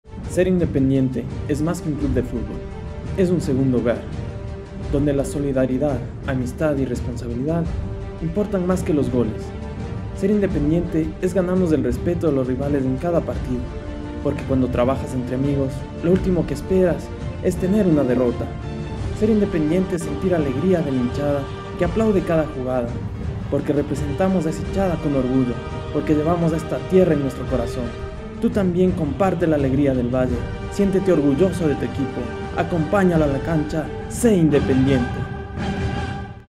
Comercial Sé Independiente (Independiente del Valle Sangolquí - Ecuador)
Propuesta de Comercial para Independiente del Valle (Proyecto de académico graduación Magister en Mercadotecnia)
Idea Original: Ramiro Cueva Ch.; Pablo Trujillo
Dirección: Pablo Trujillo A.
Producción: Doize AD en colaboración con  Aya Uma
Imágenes y cámaras: Pablo Trujillo; Cristhian Moreno
PostProducción: Ramiro Cueva Photography
Agradecimiento Especial: Independiente del Valle Club Deportivo Categorías sub-12, sub14, sub 16, Equipo de Primera. Hinchas de Independiente del Valle